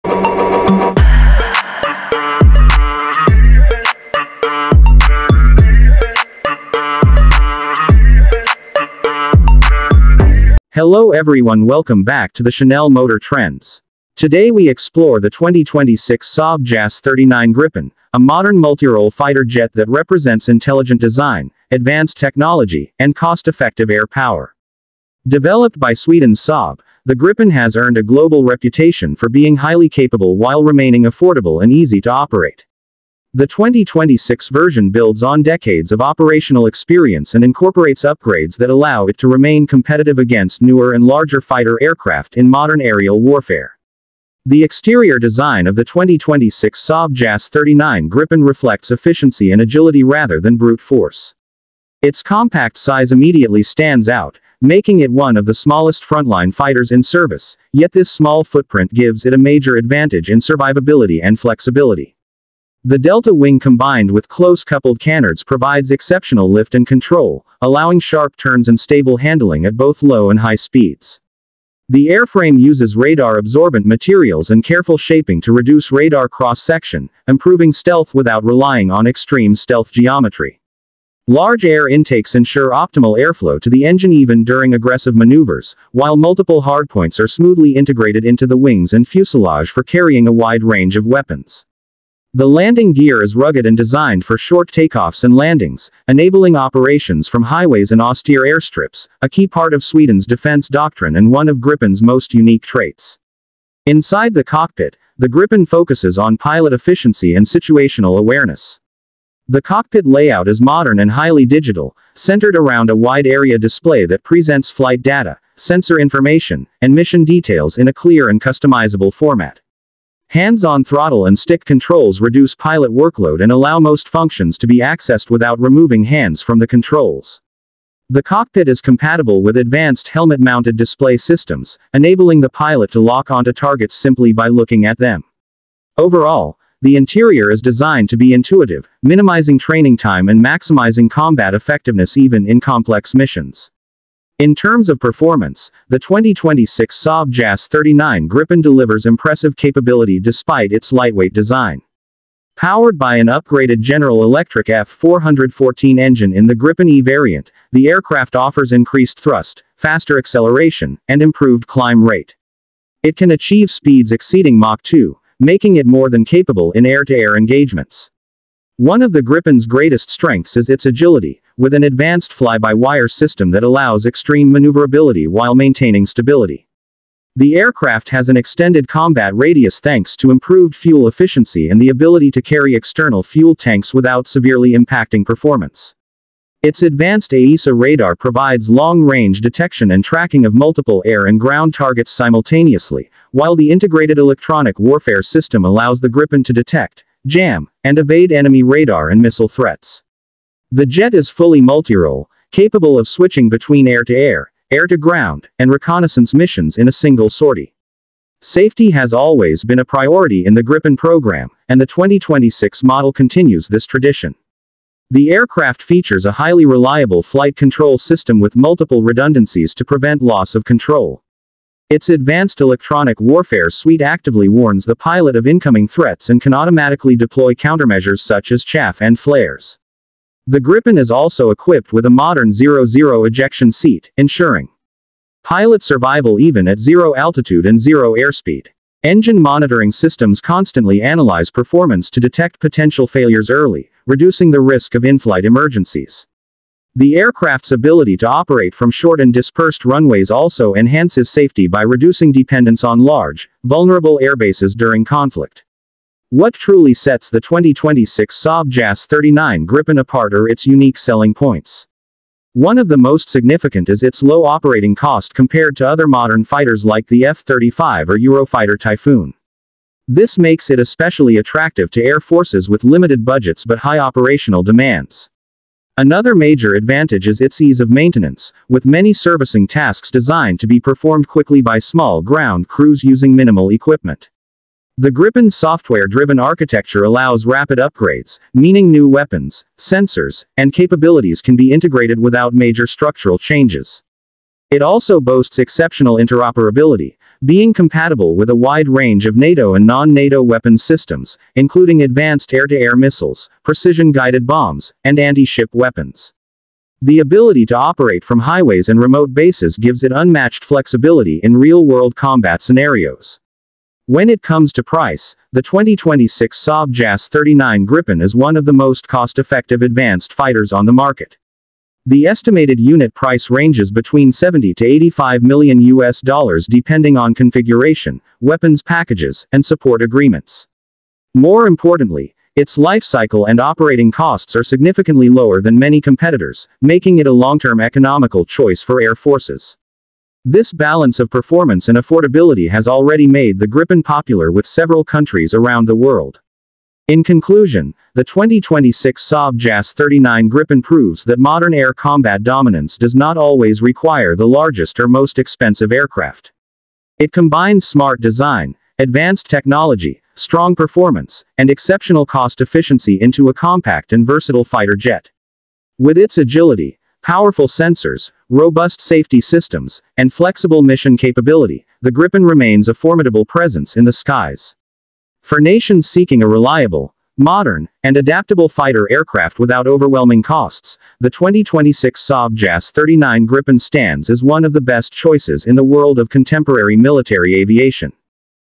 Why the 2026 Saab JAS 39 Gripen Is One of the World’s Best Fighter Jets
Discover the 2026 Saab JAS 39 Gripen, a modern multirole fighter jet designed for agility, advanced technology, and cost-effective air power. In this video, we take a detailed look at the Gripen’s exterior design, pilot-focused cockpit, powerful performance, advanced safety features, unique selling points, and estimated price. The Saab Gripen is known for its ability to operate from short runways, low operating costs, and advanced avionics, making it one of the smartest fighter jets in the world. Whether you are an aviation enthusiast, military technology fan, or curious about modern fighter aircraft, this complete review of the 2026 Saab JAS 39 Gripen will give you a clear understanding of why it remains a top choice for many air forces worldwide.